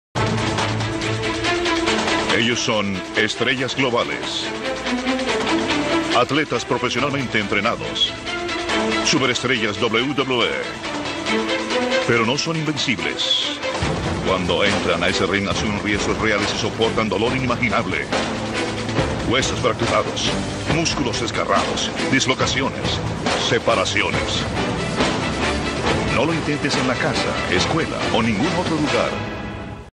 WWE No lo intentes en Casa o a menos con un profesional/ PRROWOW LUCHA LIBRE TV
Para que vean que la lucha libre es real y tiene sus riesgos